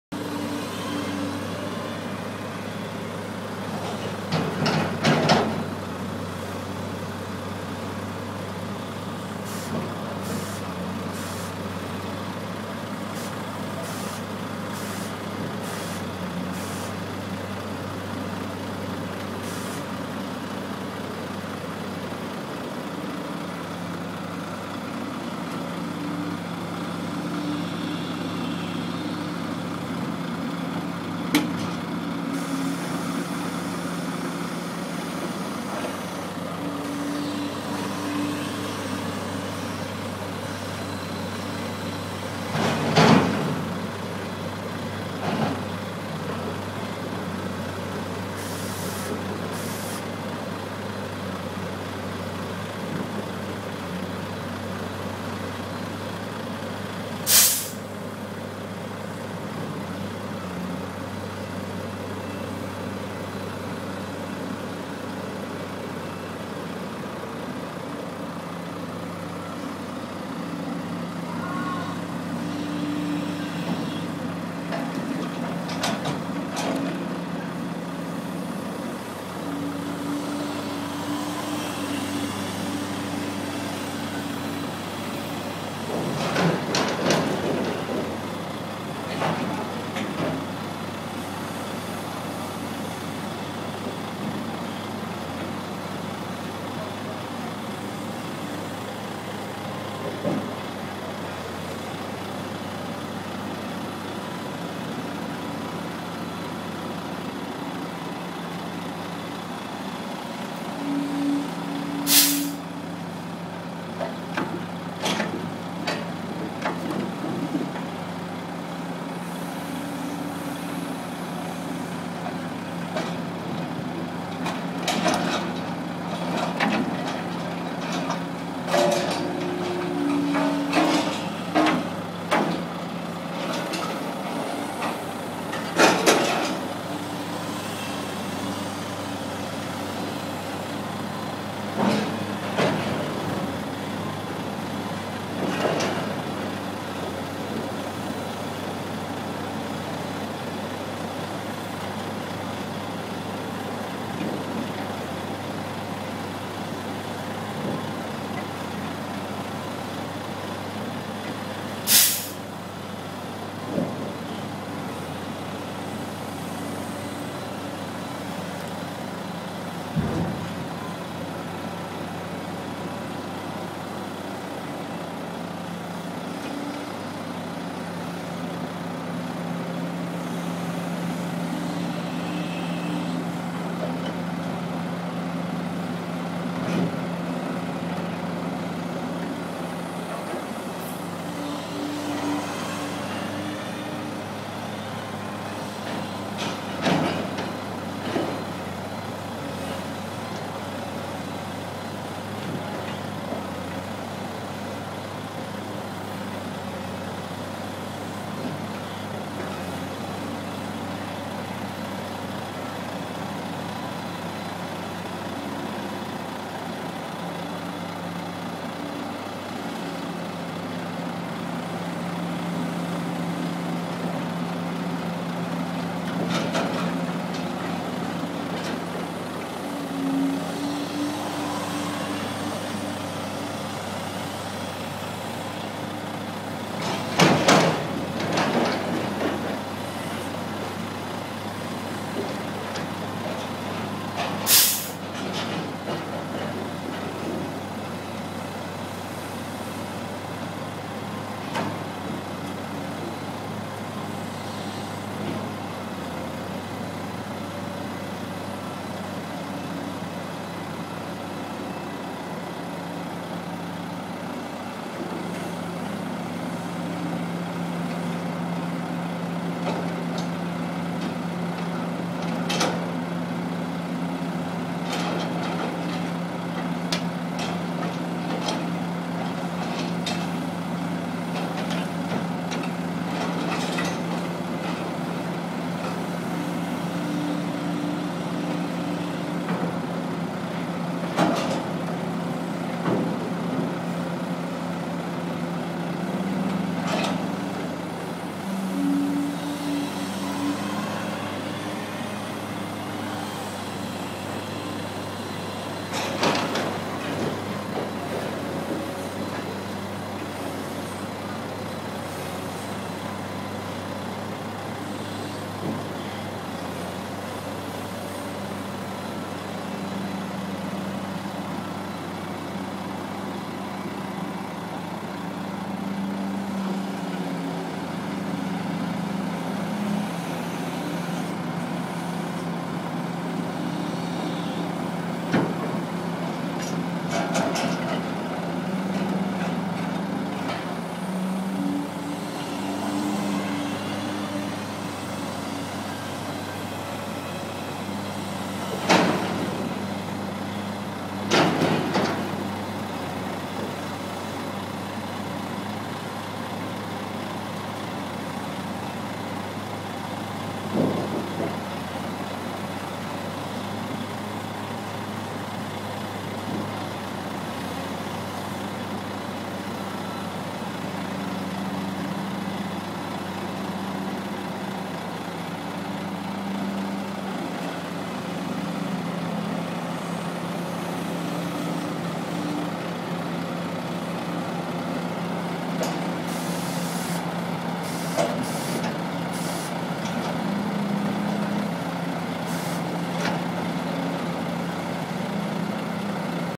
Jst Services.............The sequel................finding Nimmo
John Nimmo loading pulp at Troon Harbour.